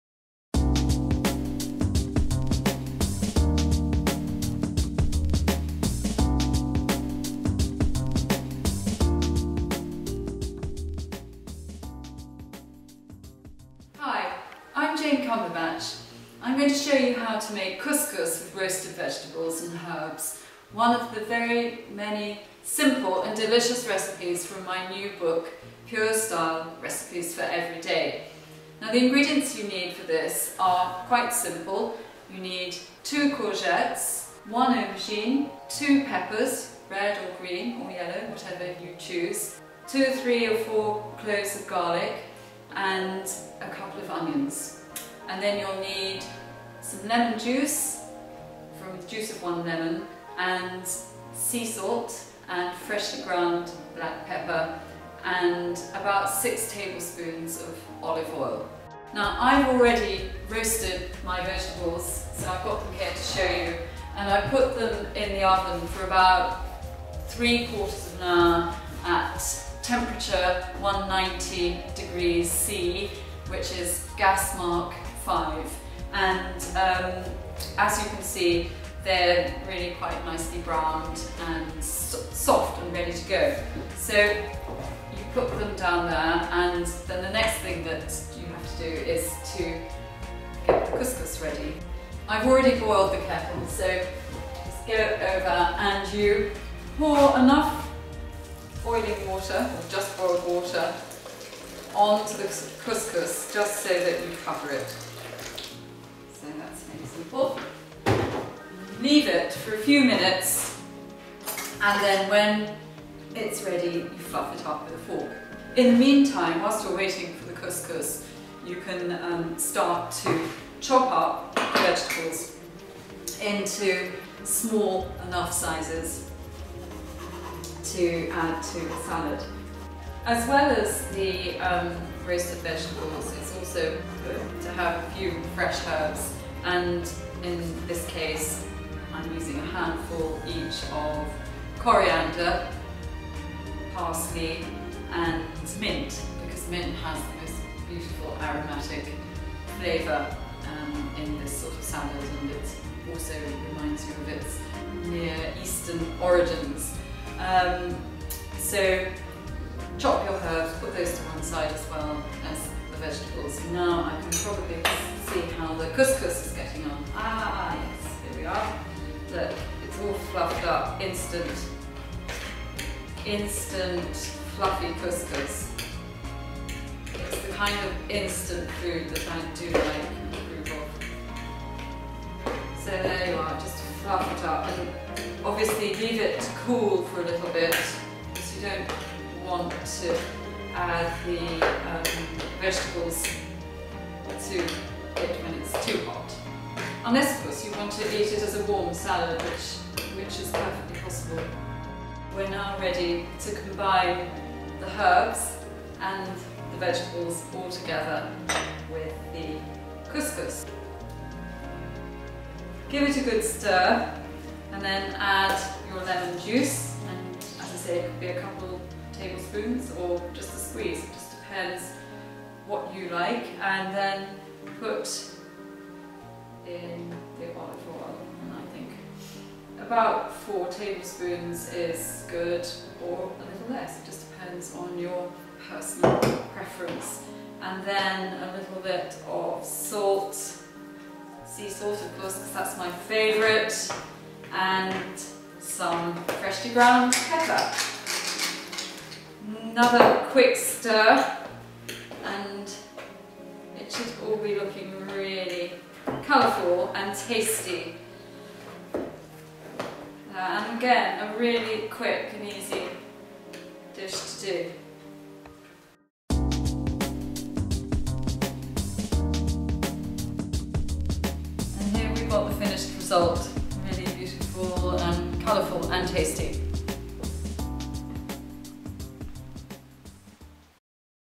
Jane Cumberbatch's Make and Do Series -- How to make couscous with roasted veg and fresh mint
Style guru Jane Cumberbatch shows you how to make couscous with roasted veg and fresh mint, one of the delicious recipes from her new book Pure Style Recipes for Everyday.